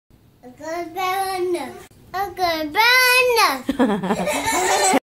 TODDLER HAS HAD ENOUGH!
Colin is being silly saying "I'VE HAD JUST ABOUT ENOUGH!" LOL!